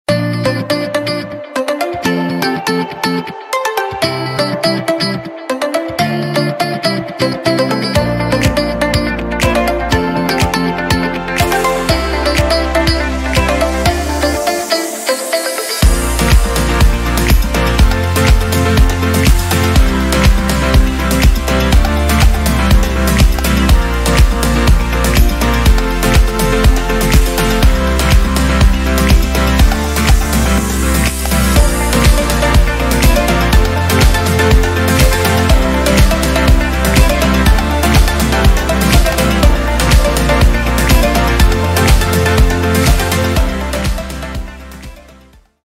45 secondes pour une peau sublime : trois bienfaits cachés du citron sur la peau ! 🍋💖
Votre peau mérite 45 secondes d'attention : découvrez les bienfaits cachés du citron en un instant ! 💖🍋